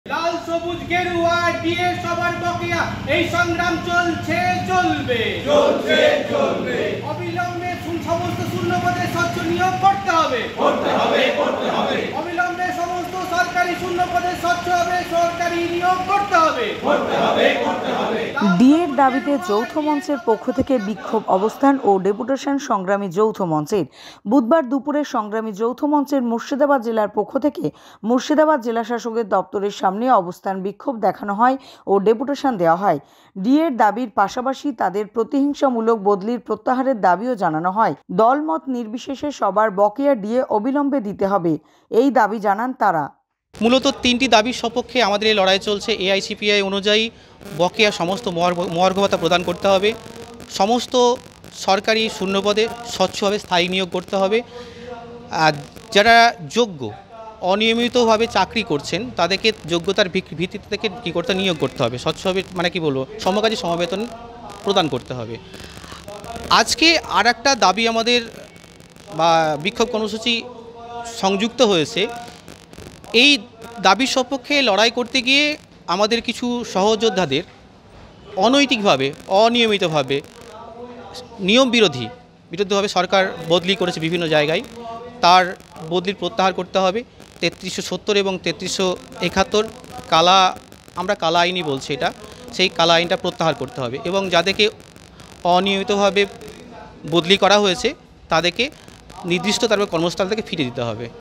DA News: ডিএ সহ তিন দফার দাবিতে সংগ্ৰামী যৌথ মঞ্চের পক্ষ থেকে বিক্ষোভ অবস্থান ও ডেপুটেশন
ডিএ এর দাবির পাশাপাশি শূন্য পদে স্বচ্ছ নিয়োগের দাবিতে এবং প্রতিহিংসা মূলক বদলির প্রত্যাহারের দাবিও জানানো হয়। দল মত নির্বিশেষে দিয়ে সবার বকেয়া ডিএ অবিলম্বে দিতে হবে।